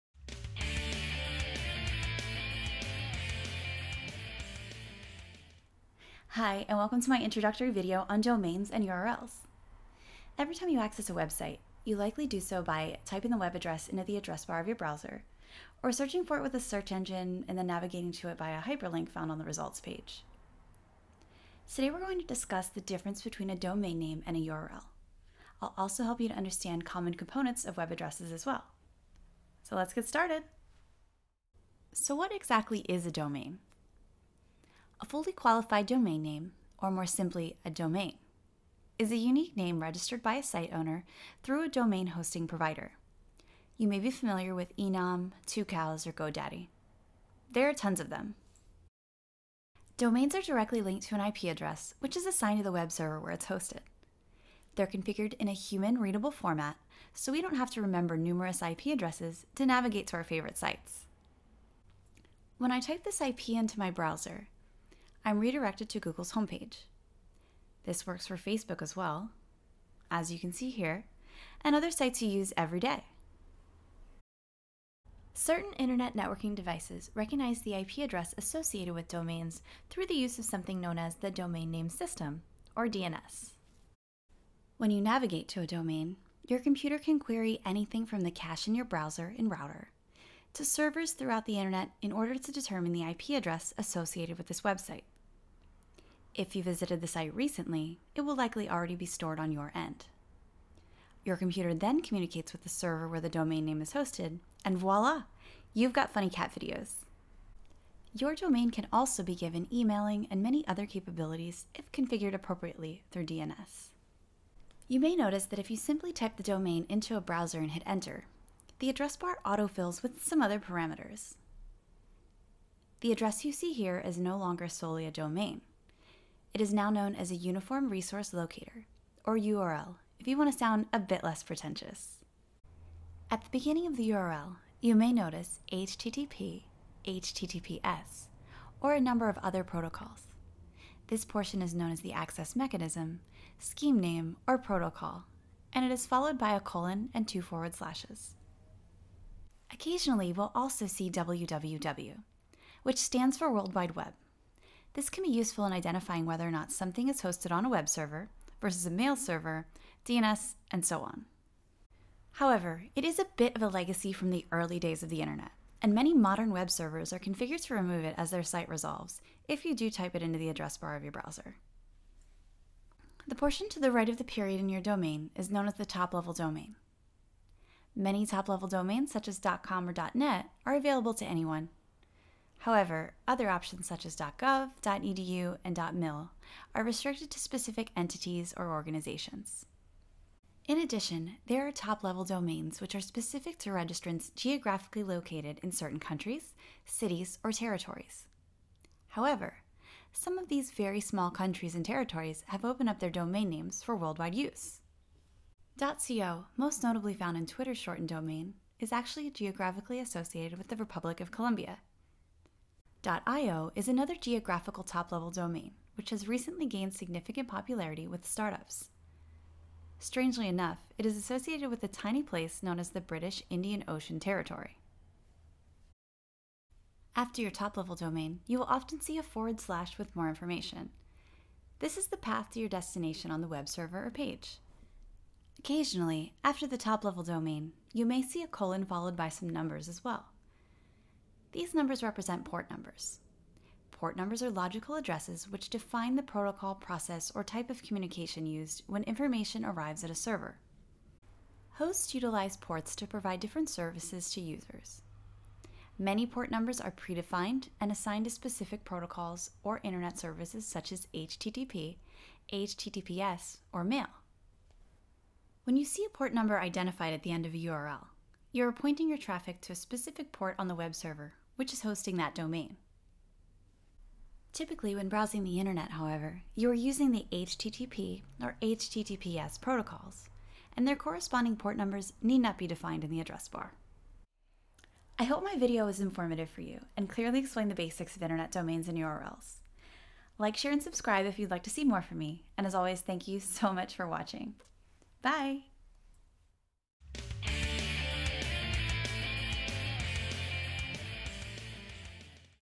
How to read a URL
Have you ever been curious about what all of that stuff means in your browser's address bar? Check out this video!